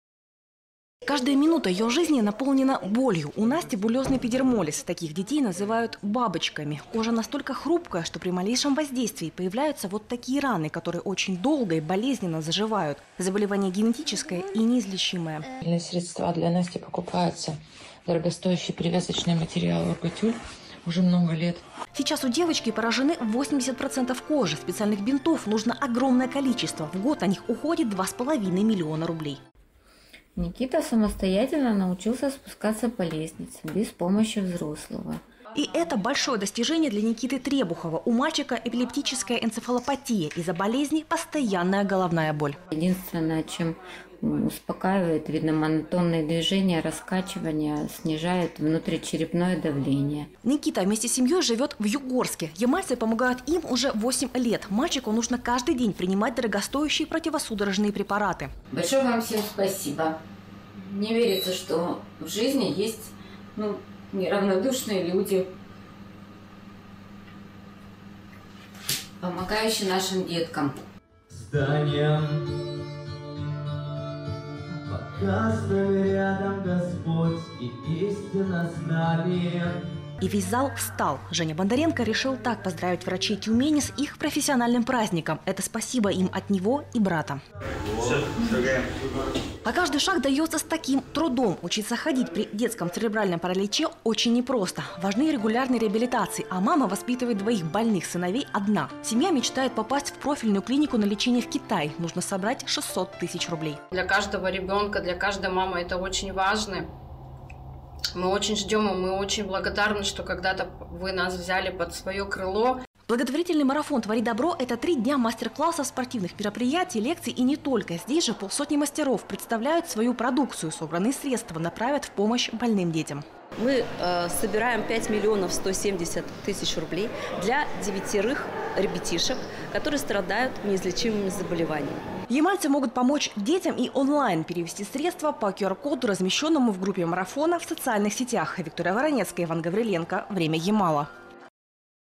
В Новом Уренгое готовятся к благотворительному марафону «Твори добро»
Уже больше 10 лет ямальцы помогают тяжело больным детям. В Новом Уренгое идут последние приготовления к традиционному благотворительному марафону «Твори добро». В этом году необходимо собрать 5 миллионов 170 тысяч рублей. Виктория Воронецкая расскажет, для чего необходима такая сумма.